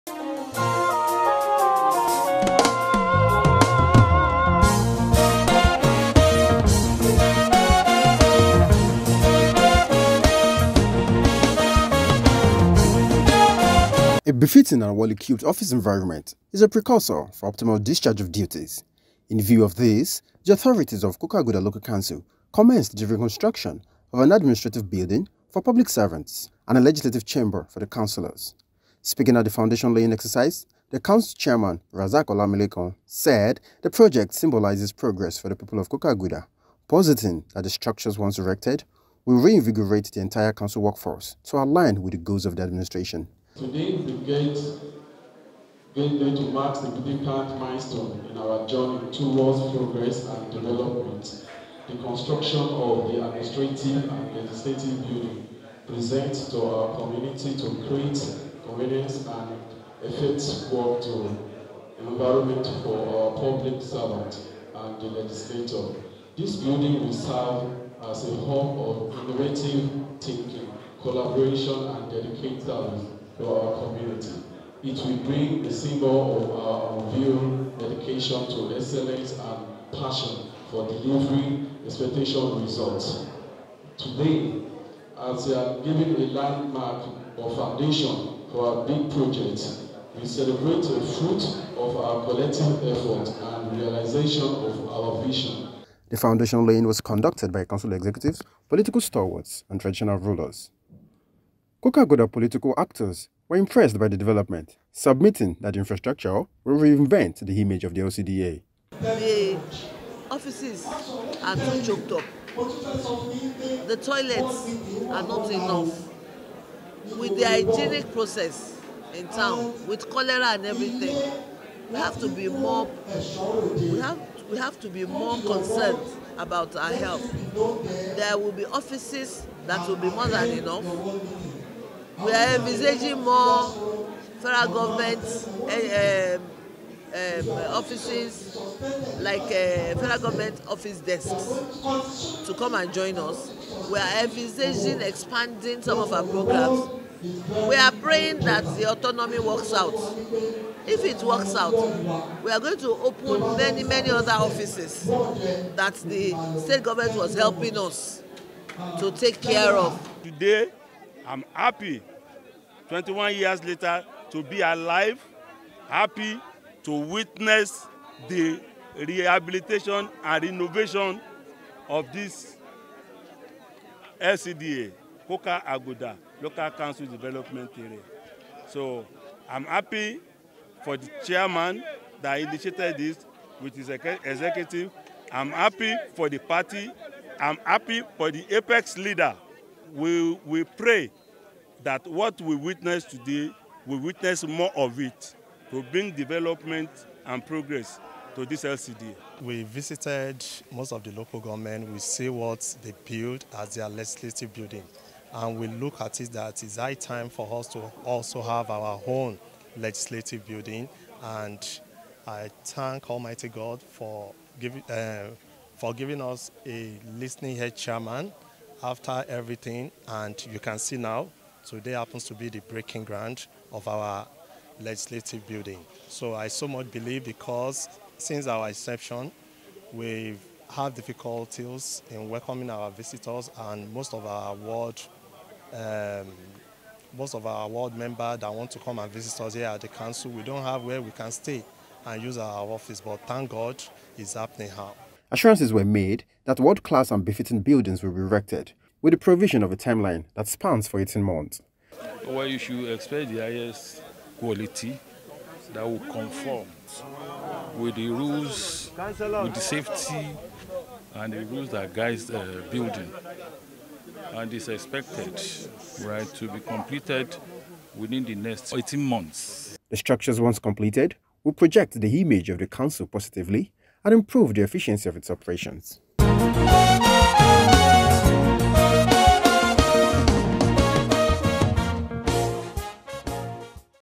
COKER AGUDA LCDA RECONSTRUCTS ADMINISTRATIVE, LEGISLATIVE BUILDING
A befitting and well-equipped office environment is a precursor for the optimal discharge of duties.

Given this, the authorities of Coker Aguda local council commenced the reconstruction of an administrative building for public servants and a legislative Chamber for the councils.

Speaking at the foundation laying exercise, the council chairman, Rasaq Olamilekan, said the project symbolizes progress for the people of Coker Aguda, positing that the structures once erected will reinvigorate the entire council workforce to align with the goals of the administration.